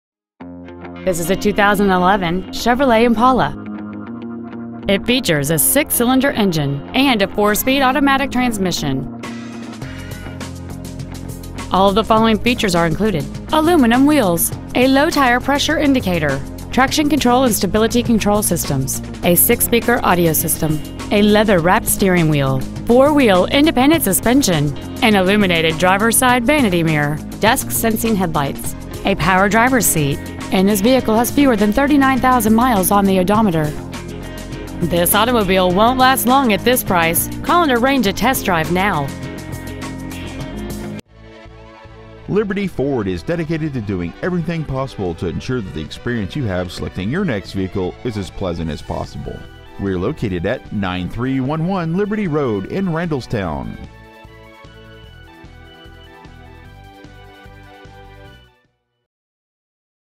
Pre-Owned 2011 CHEVROLET IMPALA Randallstown MD
We have been honored to serve the Randallstown MD area , we promise that your experience at our dealership will exceed your expectations !

 Year : 2011
 Make : CHEVROLET
 Model : IMPALA
 Engine : 3.5L V6 12V MPFI OHV FLEXIBLE FUEL
 Trans . : 4-SPEED AUTOMATIC
 Exterior : SILVER ICE METALLIC
 Miles : 38,579
 Interior : EBONY
 Stock : PA2682